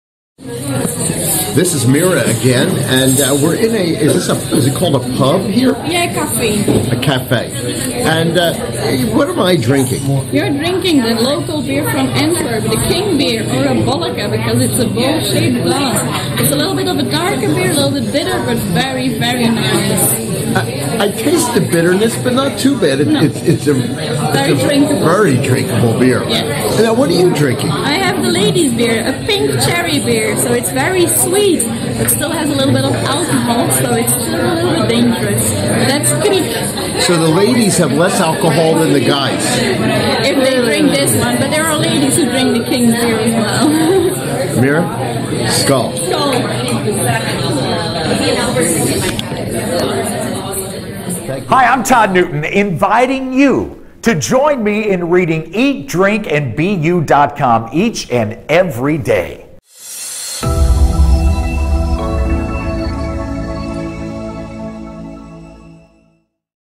An Eat Drink And Be You Beer Indulgence in Antwerp Belgium, Den Bengel Cafe
Beer Tasting in Antwerp Belgium Sampling a Bolleke and  Lambic Kriek - Cherry Beer in Den Bengel Cafe, Antwerp Belgium. Eat Drink and Be You